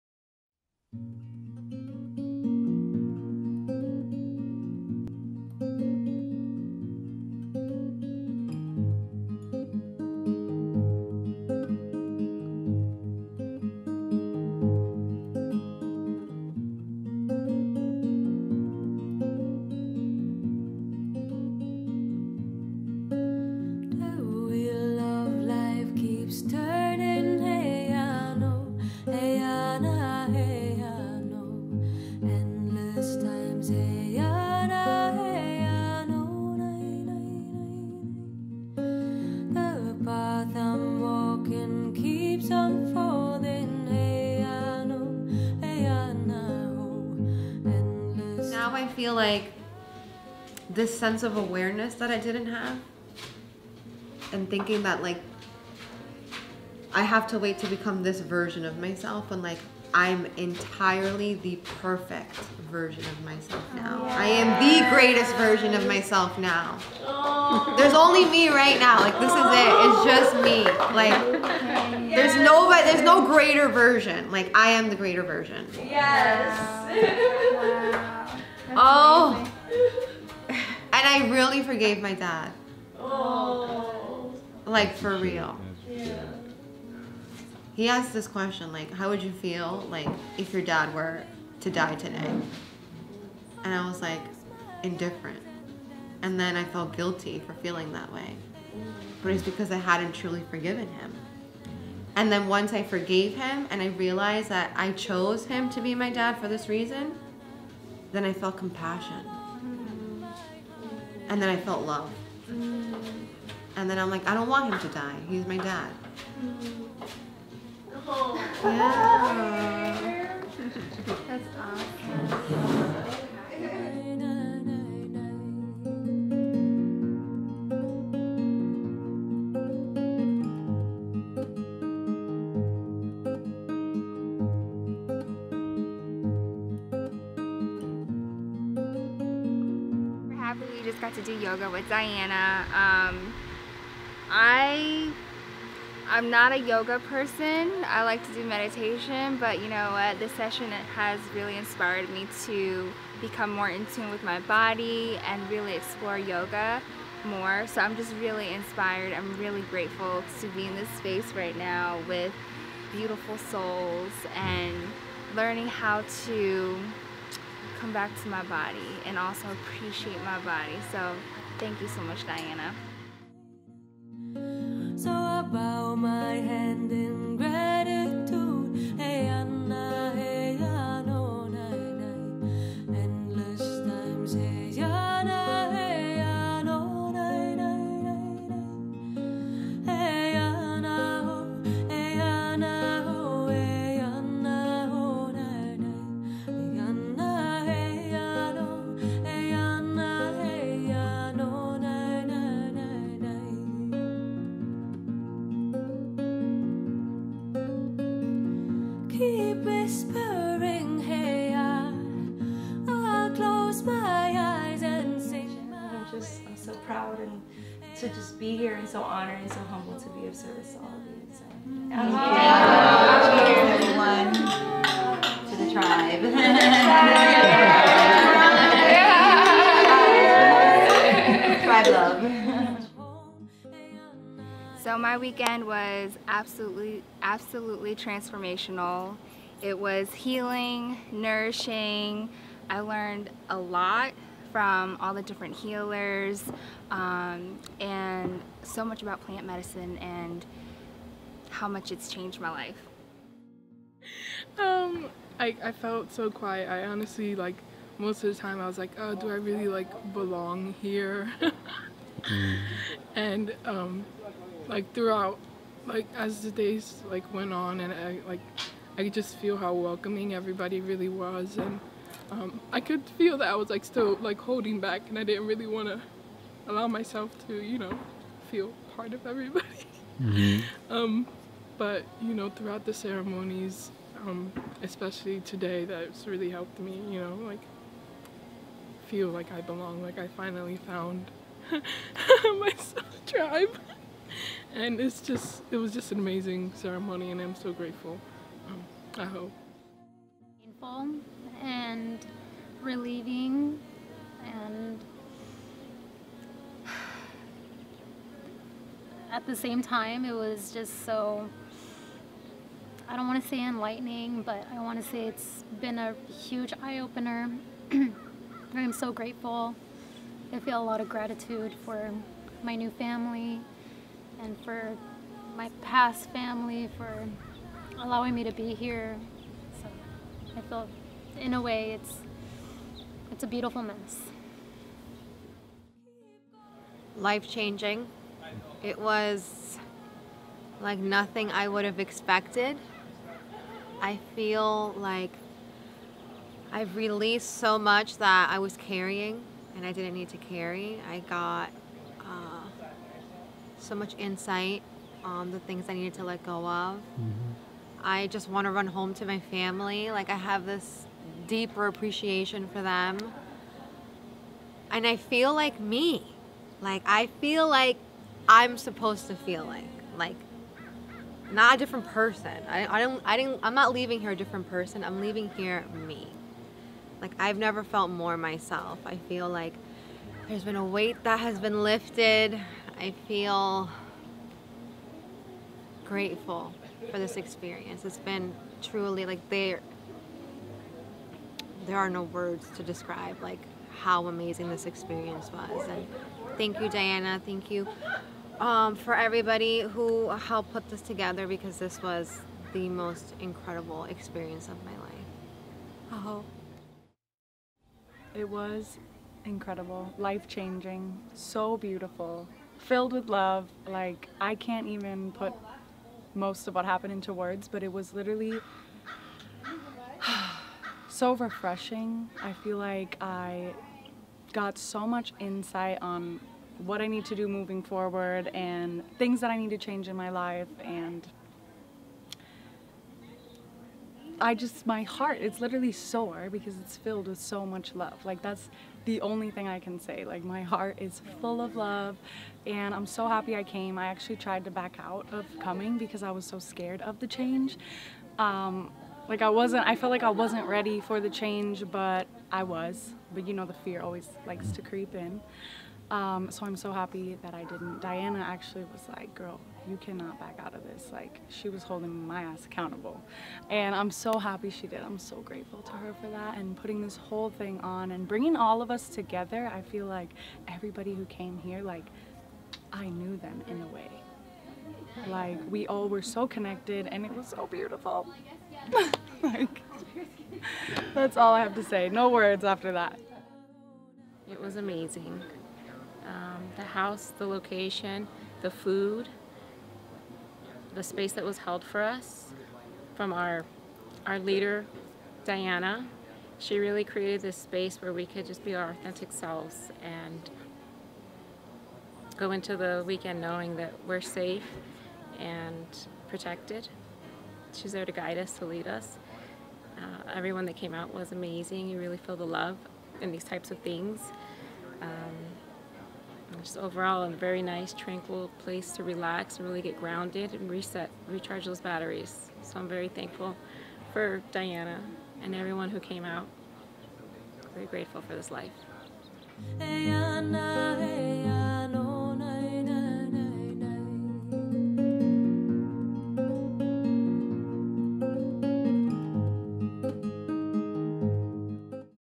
The Sacred Solstice Retreat by Diana Min The Urban Indigo
The Sacred Solstice Retreat is an intimate space for growth, for healing and for deep soul transformation.

To inquire about attending the next retreat, follow Diana Min on Instagram @theurbanindigo